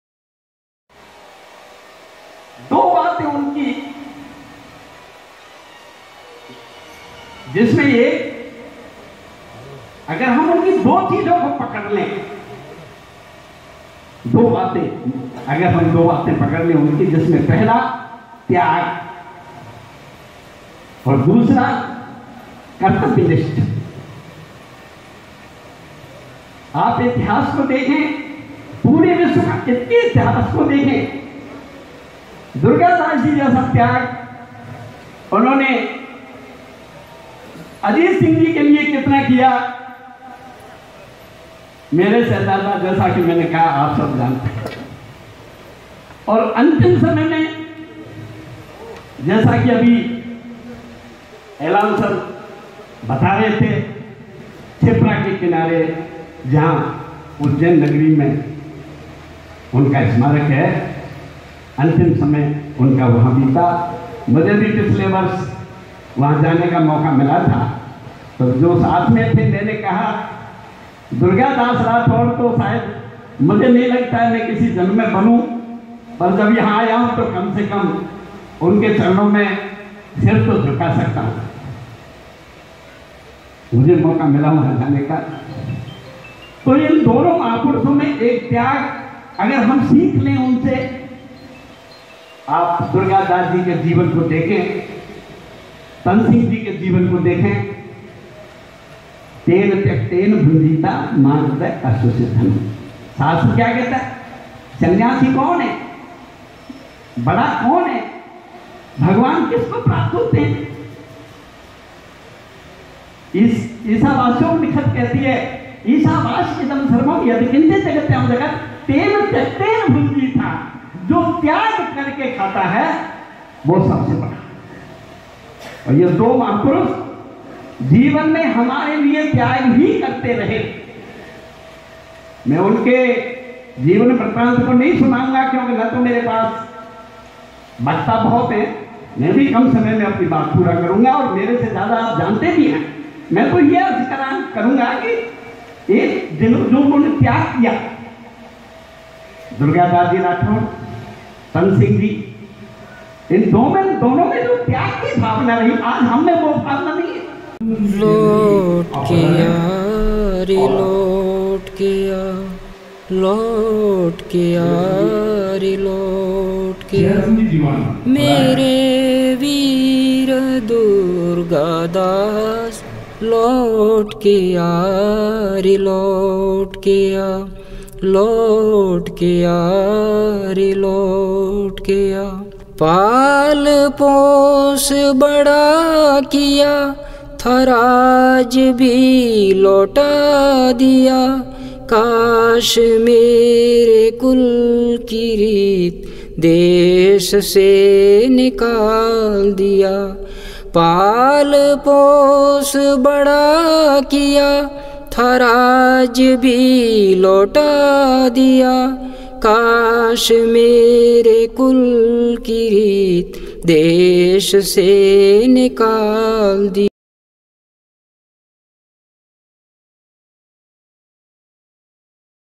वरिया महंत नारायण भारती महाराज ने वीर दुर्गादास जयंती पर क्या कहा देखे वीडियो में।।
वीर शिरोमणी दुर्गादास जयंती समारोह ,जीवाणा( जालोर)